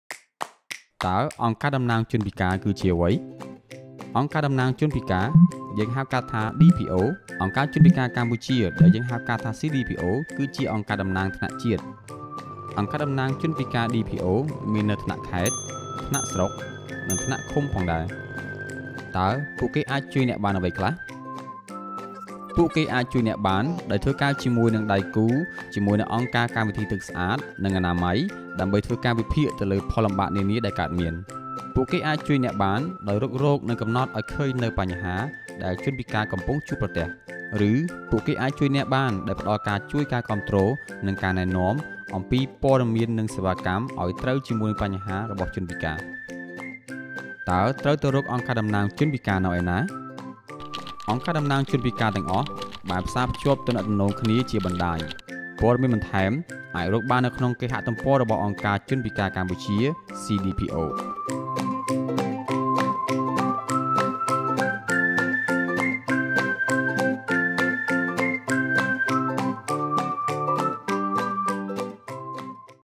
How to partner with a DPO | WaterAid (English version)
An overview of Disabled People's Organisations (DPOs) in Cambodia, and the different levels of DPO that represent people with a disability. DPOs can provide information and support to identify problems faced by people with a disability.